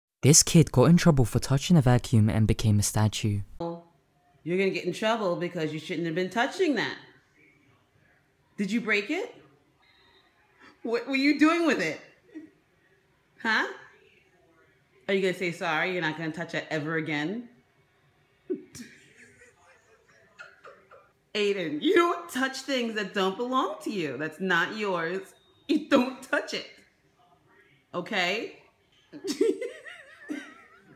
Kid Becomes A STATUE😂😂😂
Kid Becomes A STATUE when he is in trouble for using a vacuum cleaner